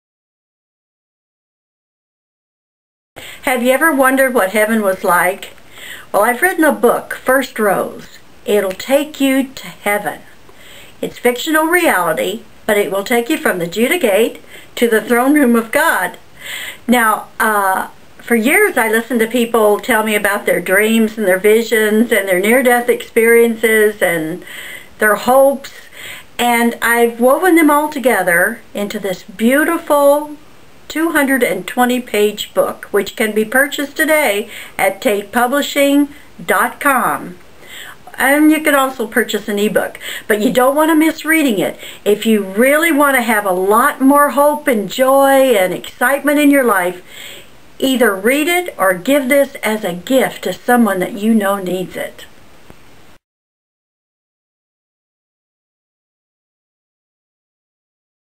FirstRose.wmv
About the book FIRST ROSE which is a complete tour of the Heavenly city.